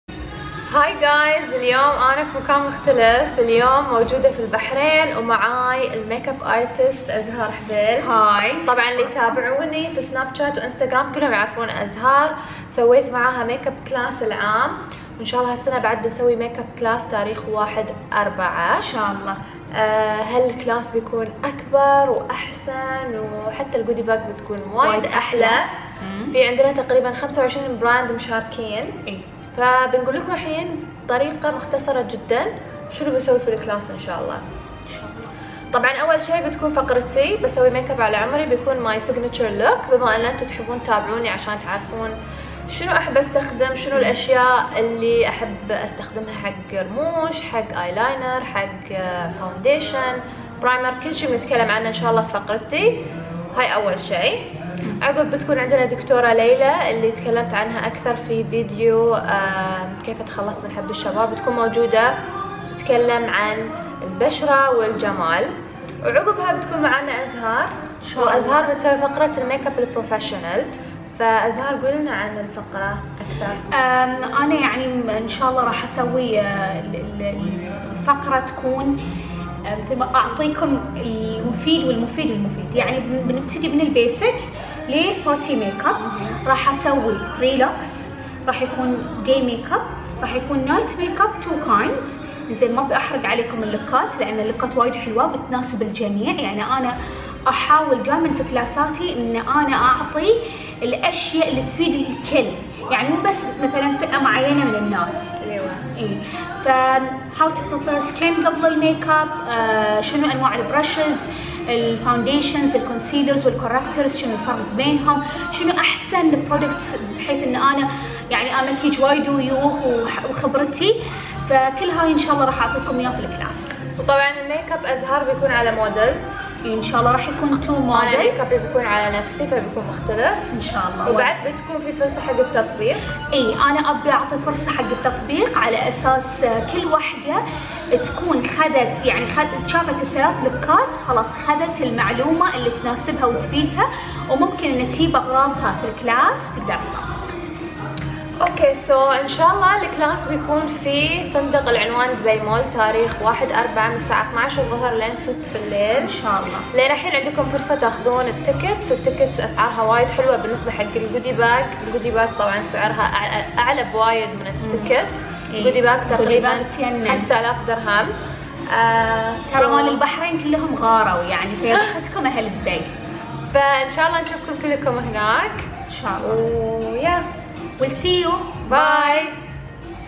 إعلان كلاس الميك أب في دبي | AlReem Dubai Makeup Class
المكان: فندق العنوان دبي مول
الزمان: السبت ١ أبريل ٢٠١٧
للحجز واتساب: ٠٥٥٩١٥٥٩٩٧
هدايا بقيمة ٥٠٠٠ درهم لجميع المشتركات